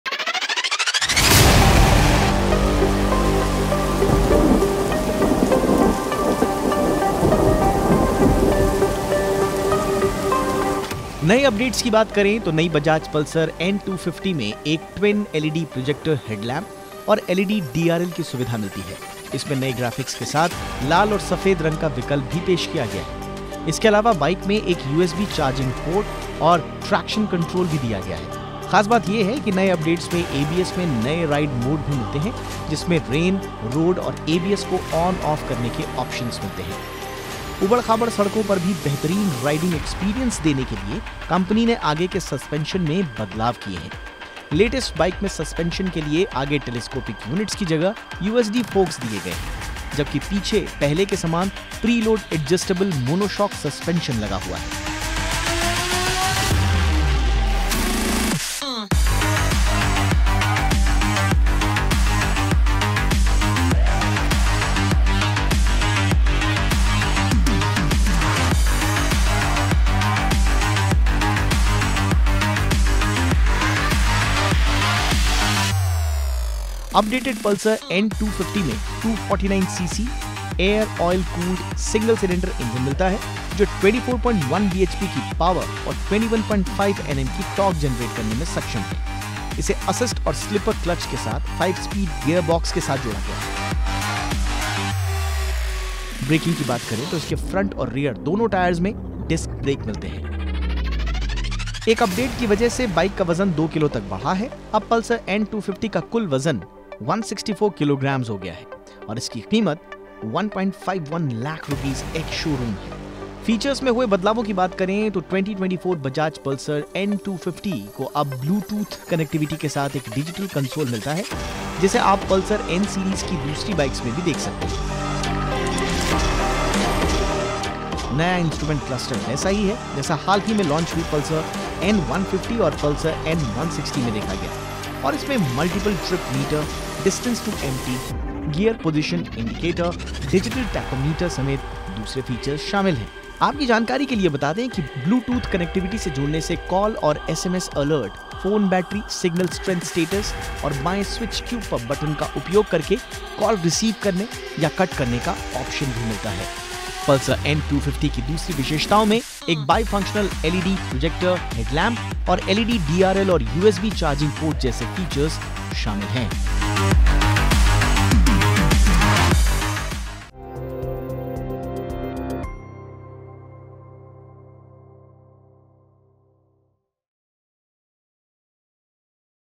Zeegnition | कैसी है 2024 में लॉन्च हुई Pulsar N250?
देखिए फुल रिव्यू